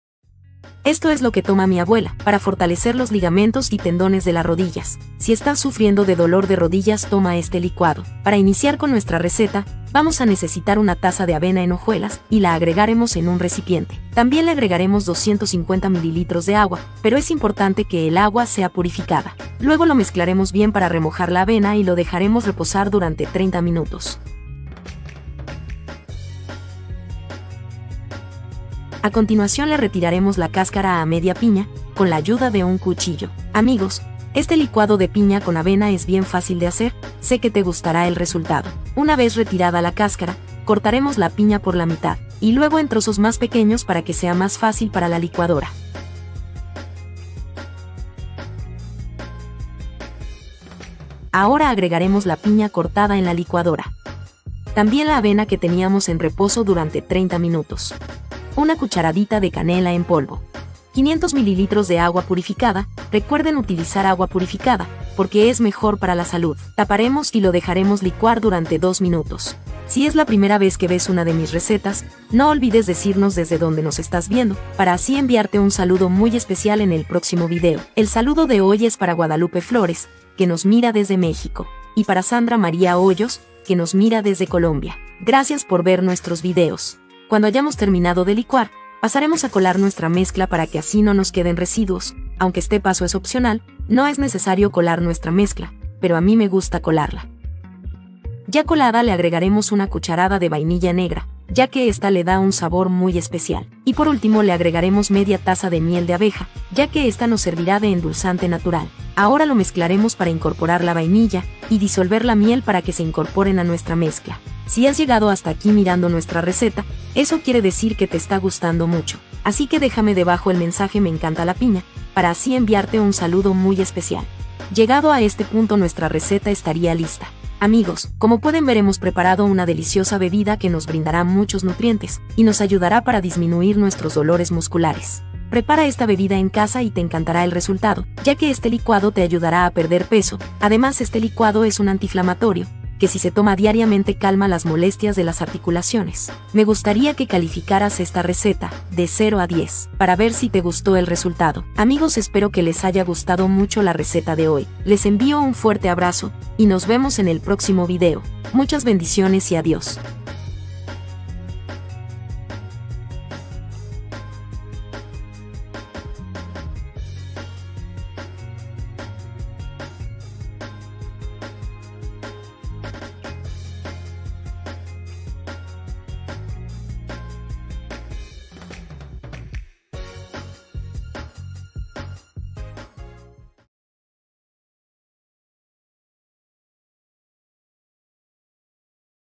Mezcla piña con avena y te sorprenderá el resultado! Mundo De Recetas
Prepara este delicioso jugo de piña  encantará

Muchas bendiciones!